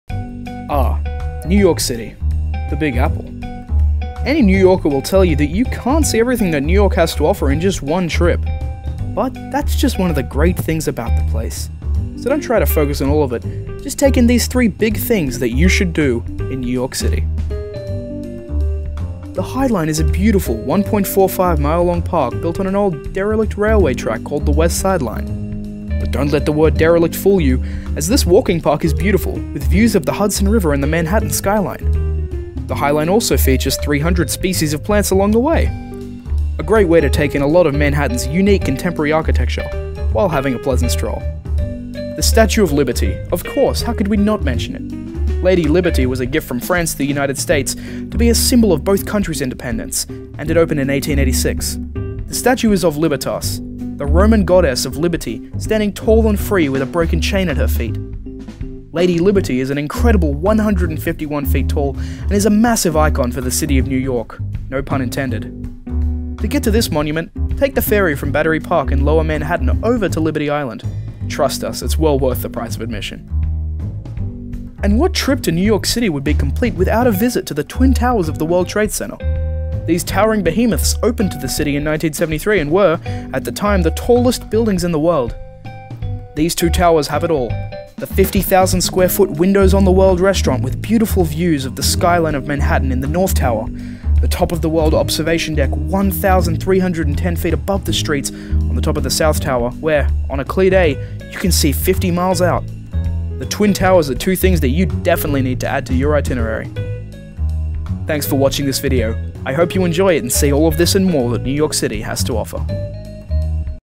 Things To Do In: New York City 2017
Ah, New York City. The Big Apple. Any New Yorker will tell you that you can't see everything that New York has to offer in just one trip. But that's just one of the great things about it. So don't try to focus on all of it, just take in these three big things that you should do in New York City.

"Pre 9/11 World Trade Center - Aug. 18,19 2001- A video of the Twin Towers before the attacks - WTC" - by HistoryofTek: